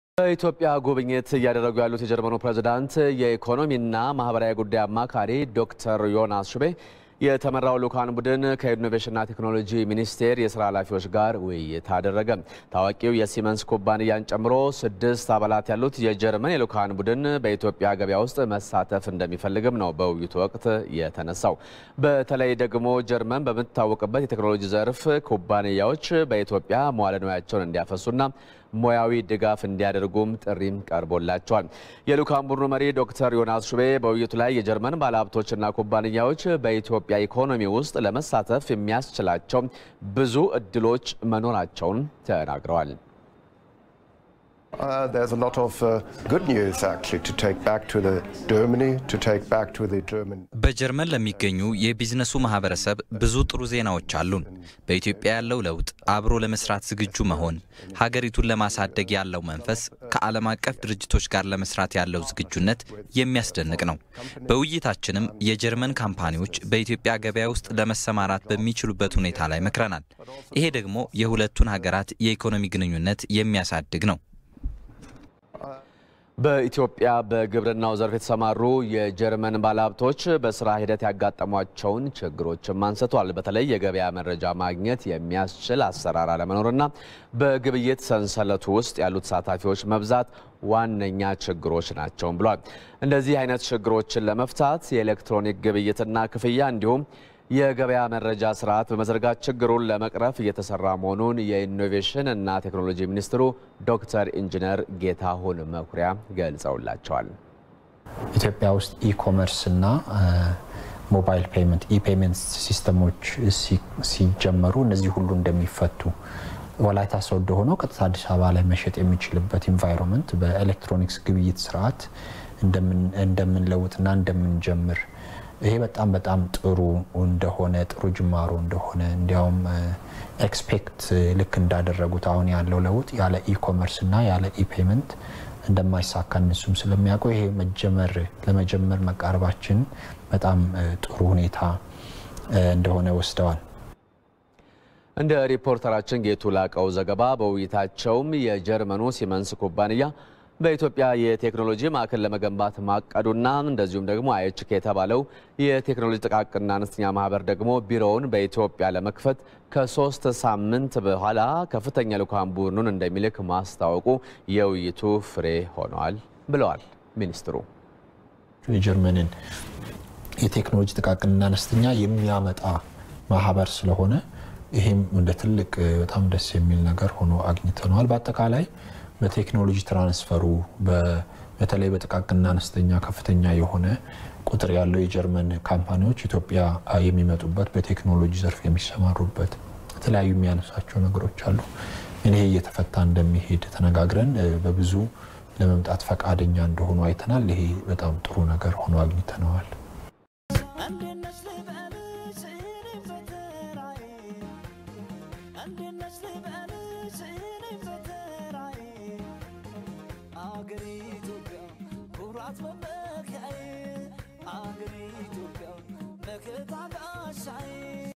German companies including Volkswagen and Siemens embarking to invest in Ethiopia
German companies including automaker Volkswagen and conglomerate Siemens embarking to invest in Ethiopia